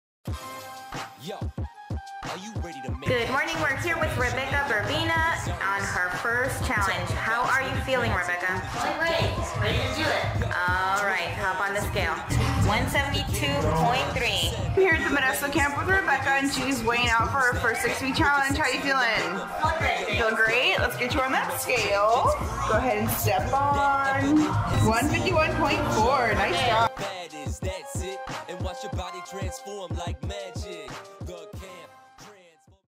Modesto Weight Loss Fitness 6 Week Challenge Results - Rebecca Berbena
We will now check the result of Rebecca's 6 week weight loss challenge!